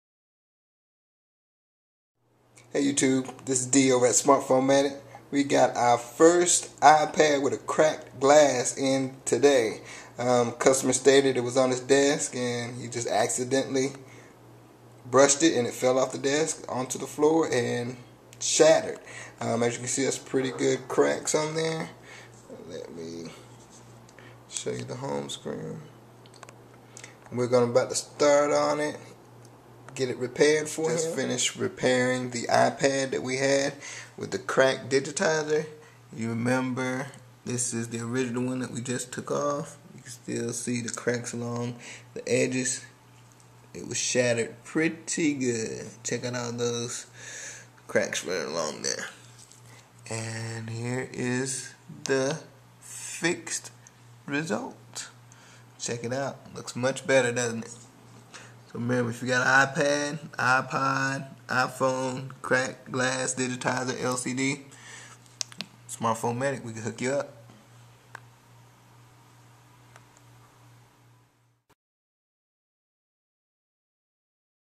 iPad Cracked Glass Repair By SmartPhone Medic
This was our first iPad cracked glass repair.  We were surprised that we already received one for repair since they just came out in April.